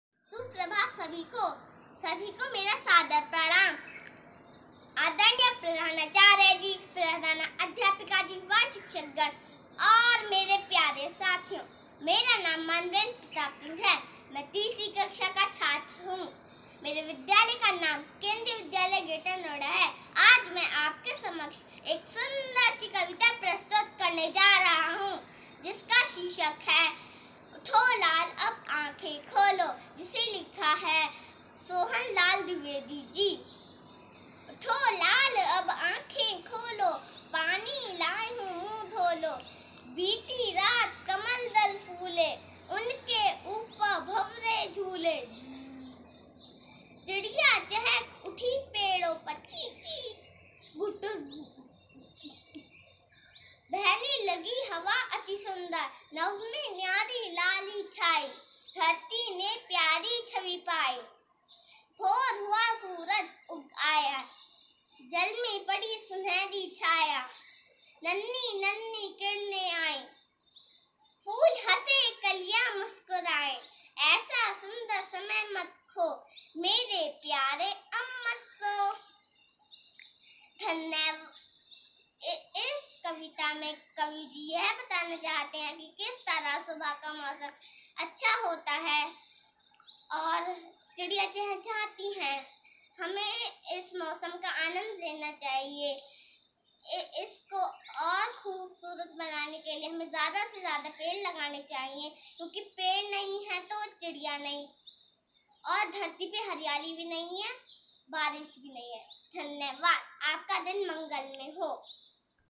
मानवेंद्रप्रतापसिंह उठोलालअबआँखेंखोलो श्रीसोहनलालद्विवेदी
उठो लाल अब आँखें खोलो,
पानी लायी हूँ मुंह धो लो।

बीती रात कमल दल फूले,
उसके ऊपर भँवरे झूले।

चिड़िया चहक उठी पेड़ों पे,
बहने लगी हवा अति सुंदर।

नभ में प्यारी लाली छाई,
धरती ने प्यारी छवि पाई।

भोर हुई सूरज उग आया,
जल में पड़ी सुनहरी छाया।

नन्ही नन्ही किरणें आई,
फूल खिले कलियाँ मुस्काई।

इतना सुंदर समय मत खोओ,
मेरे प्यारे अब मत सोओ।

— सोहनलाल द्विवेदी